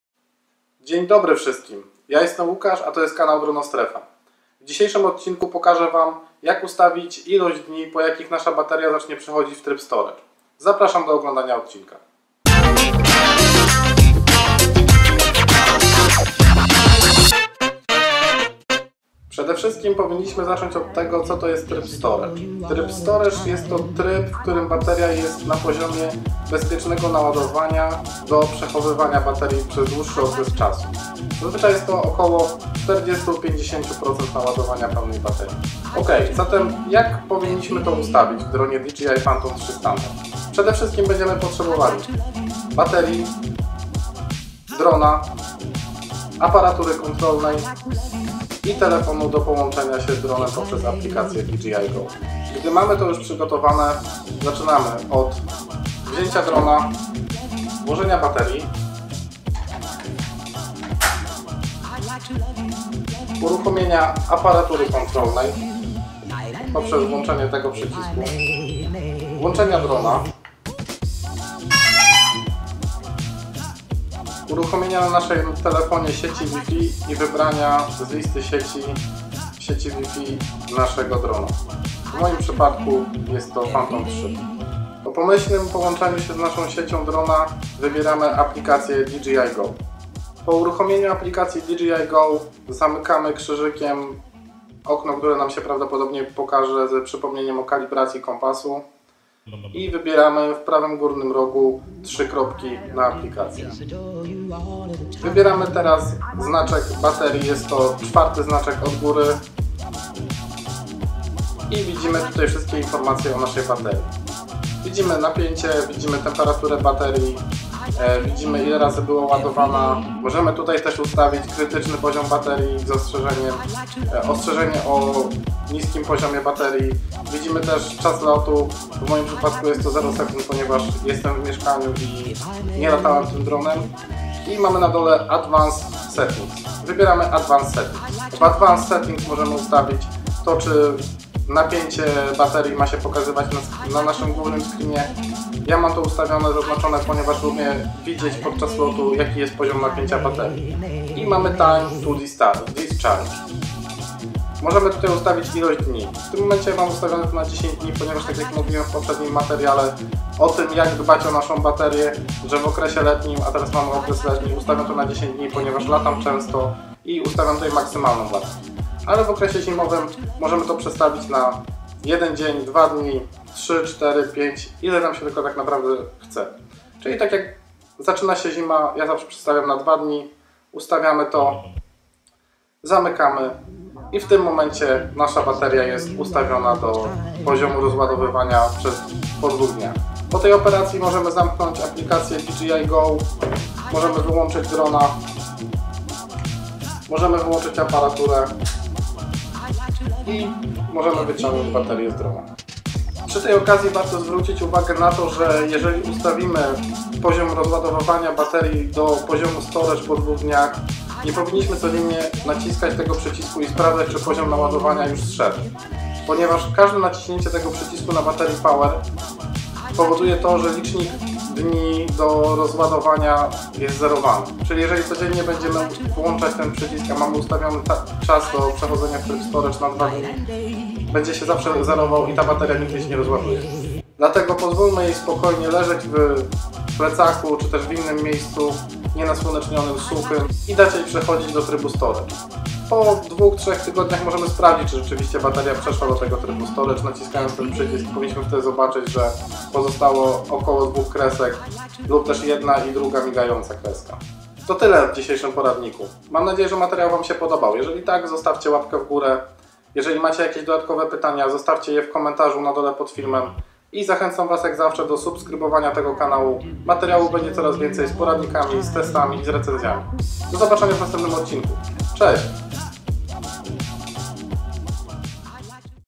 Tryb Storage - Jak ustawić w Phantom 3?
W dzisiejszym odcinku pokazuję jak ustawić w aplikacji DJI GO ilość dni po jakich nasza bateria zacznie przechodzić w tryb storage.